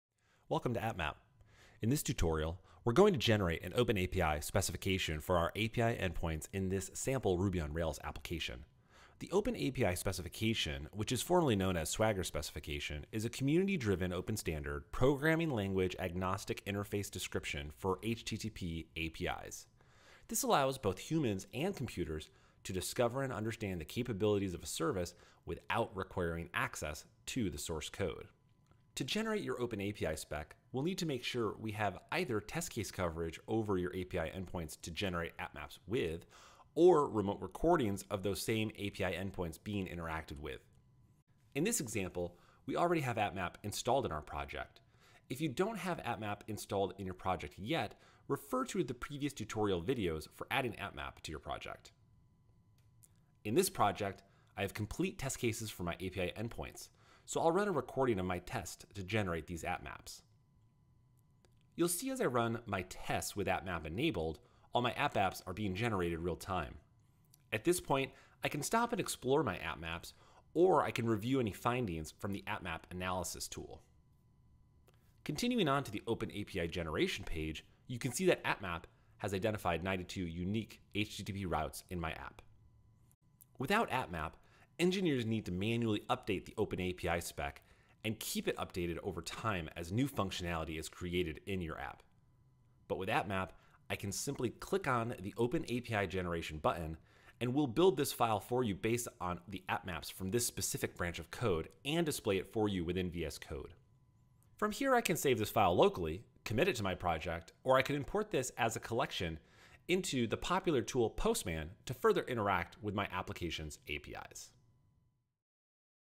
Generate an OpenAPI Spec from your AppMaps
Welcome to AppMap, in this tutorial we are going to generate an openapi specification for our API endpoints in this sample ruby on rails applications.  The OpenAPI specification, which is formerly known as Swagger Specification, is a community driven open standard, programming language-agnostic interface description for HTTP APIs. This allows both humans and computers to discover and understand the capabilities of a service without requiring access to the source code.

To generate your Open API spec, we’ll need to make sure we have either test case coverage over all your API endpoints to generate Appmaps OR remote recordings of those same API endpoints being interacted with.

In this example, we already have AppMap installed in our project. If you have not installed AppMap in your project yet, refer to our previous tutorial video for adding Appmap to your project.

In this project, I have complete test cases for my API endpoints, so i’ll run a recording of my tests to generate the appmaps.
You’ll see as I run my tests with appmap enabled, all my appmaps being generated real time.  At this point I can stop and explore my appmaps, or I can review any findings from the AppMap Analysis.

Continuing on to the OpenAPI generation page, you can see that AppMap has identified 92 unique HTTP routes in my app.

Without AppMap, engineers need to manually update the openAPI spec and keep updated over time as new functionality is created in your app .  But with appmap, we can simply click on the open API generation button, and we’ll build this file for you based on the appmaps from this specific branch of code and display it within VSCode.  From here I can save this file locally, commit it to my project, or I can import this as a collection into the popular open source tool, Postman to further interact with my application’s APIs.